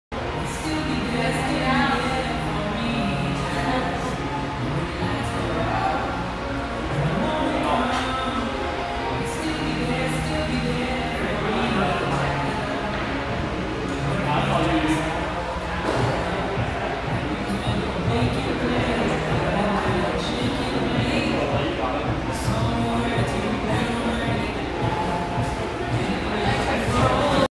Climbing ASMR part 2
Dumping all my indoor climbing videos onto my account. Just some post training junk here.